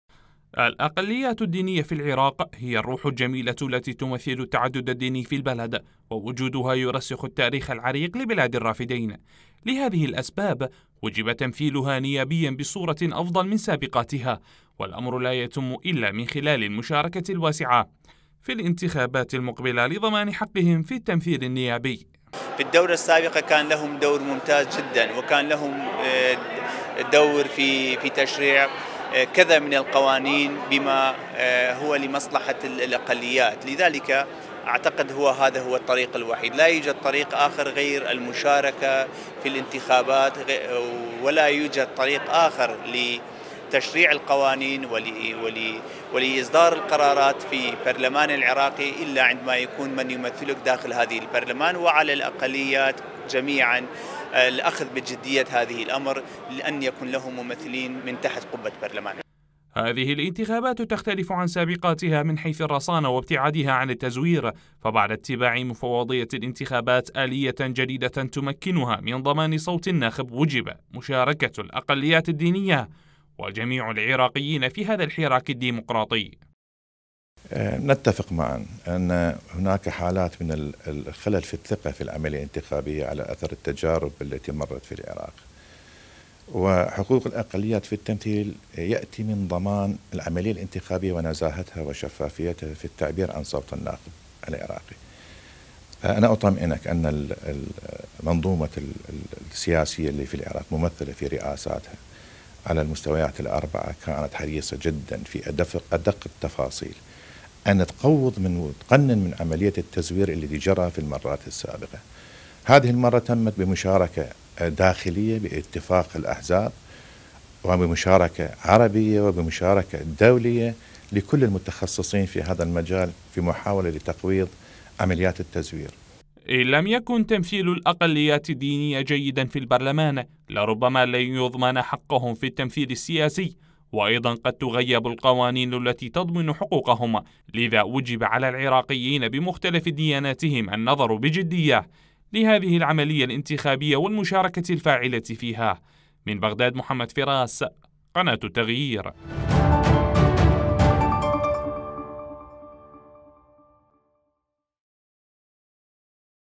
الأقليات تعتزم المشاركة بقوة في الاقتراع للحفاظ على تعددية الاطياف | تقرير
وتعتزم الاقليات الدينية في العراق في هذه الانتخابات ان تشارك بقوة بهدف الحفاظ على مكانة الديانات العراقية المتعددة وتعددية الاطياف.